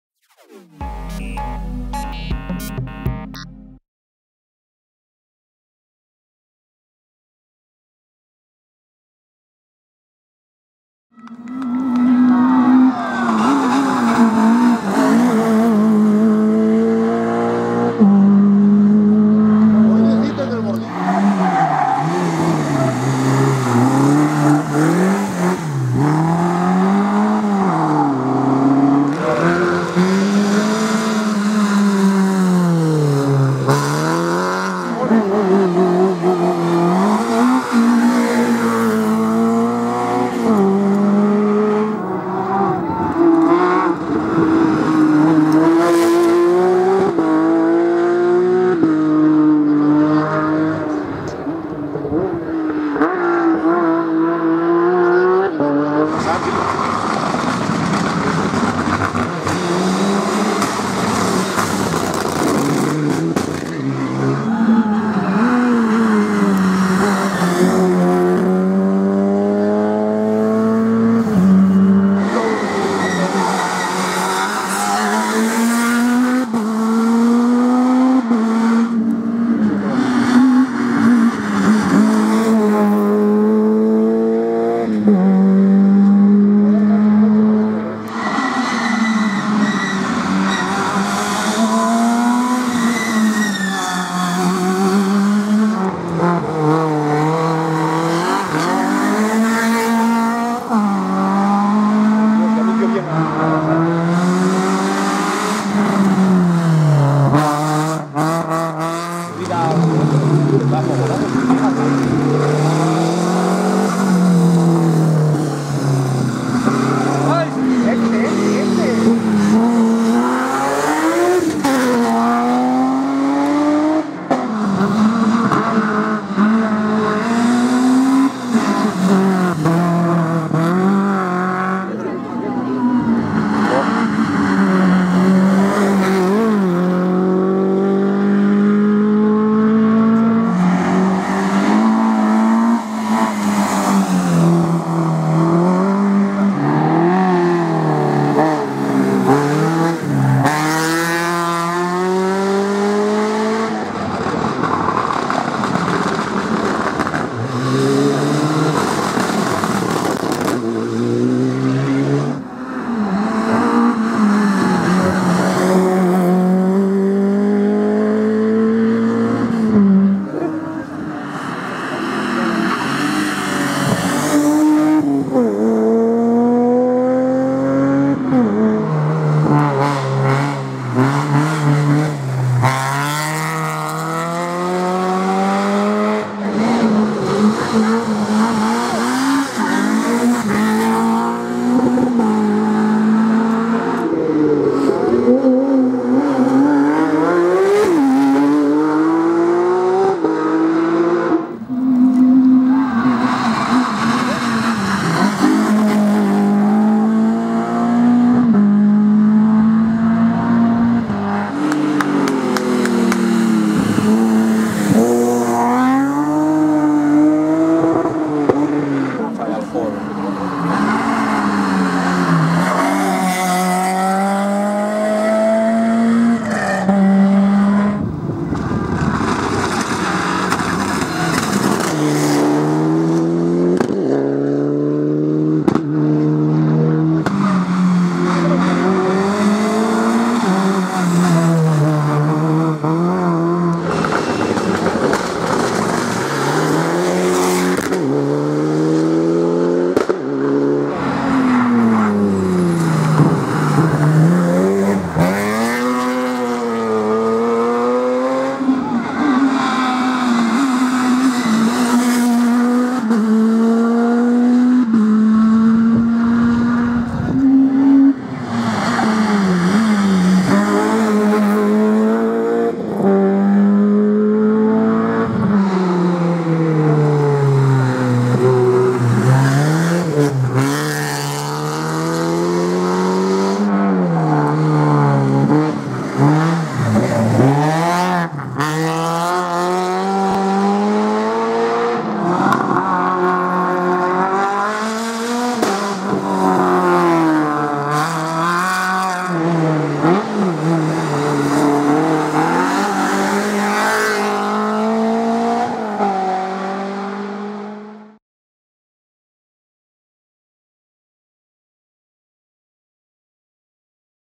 V Rallysprint Villa de Grado | 2015
A pesar de no ser los favoritos al triunfo, el trío formado por Marcelino Hevia, Agustín Ramos y el Fiat Abarth Grande Punto S2000, consiguieron la victoria en el V Rallysprint Villa de Grado, dominando de principio a fin por las carreteras mosconas.